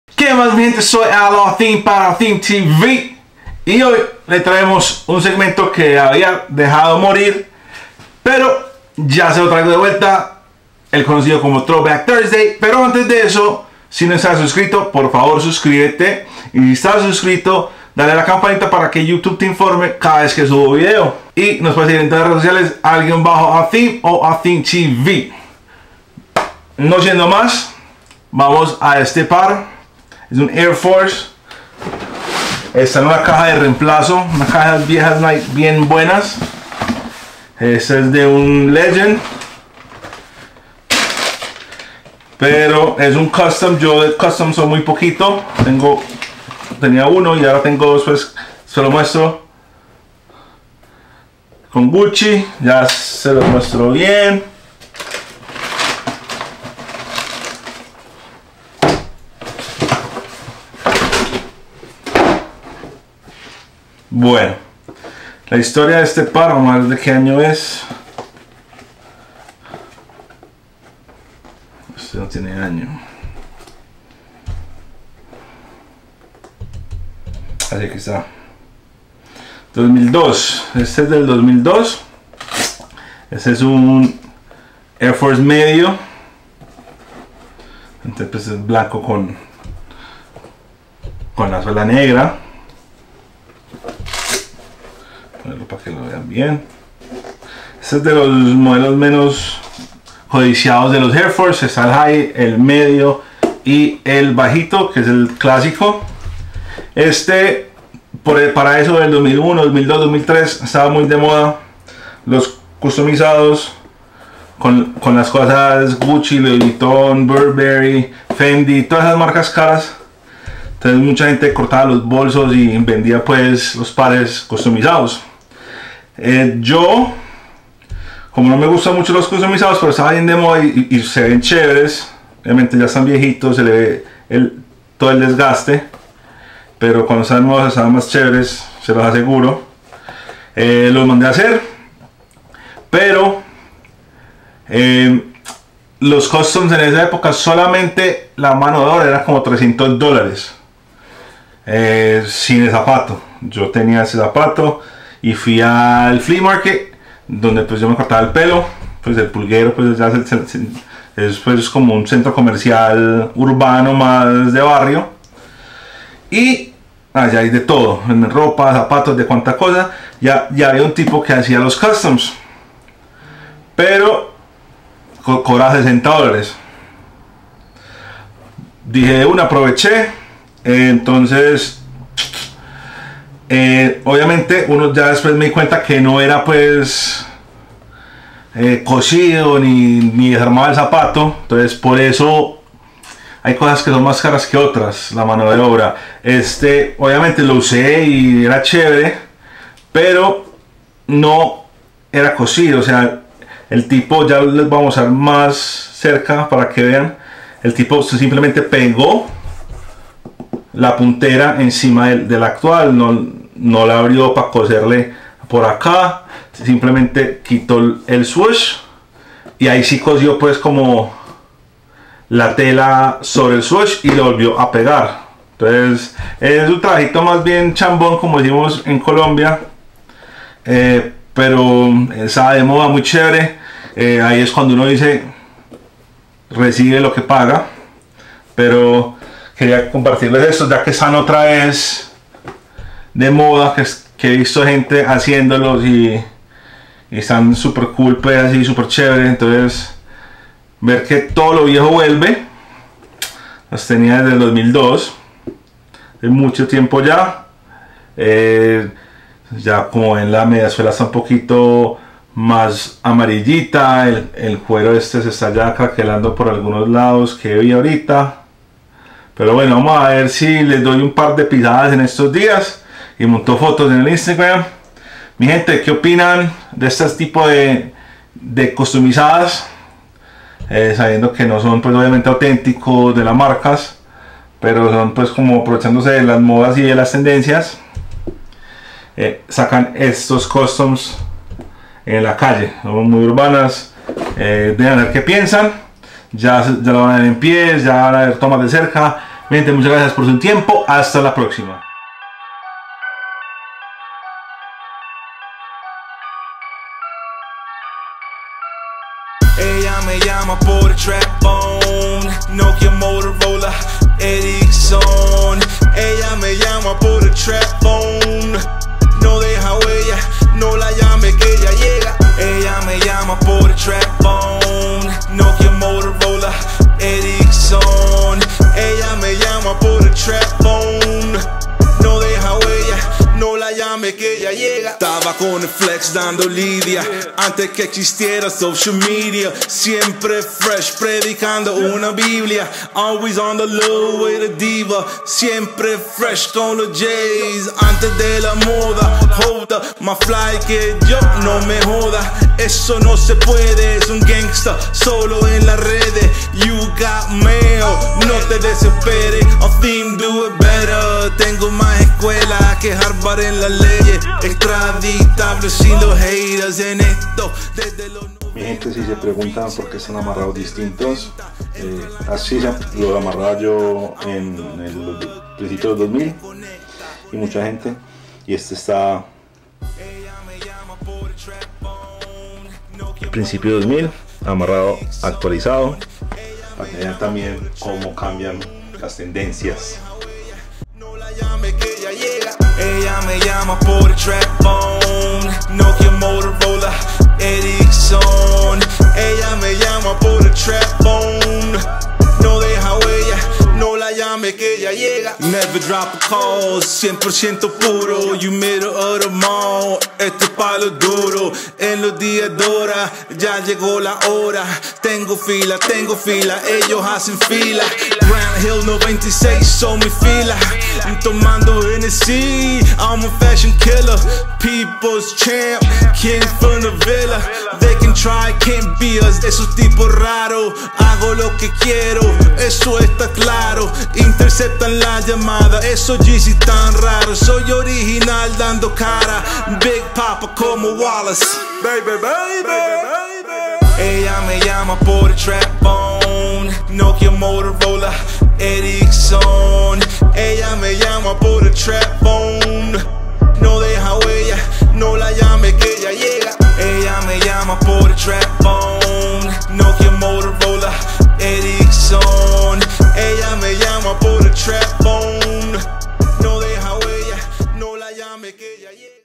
TBT Nike Air Force 1 Gucci (Custom)
Q mas mi gente les traigo un tbt de estos custom gucci air force one.

musica Al Athim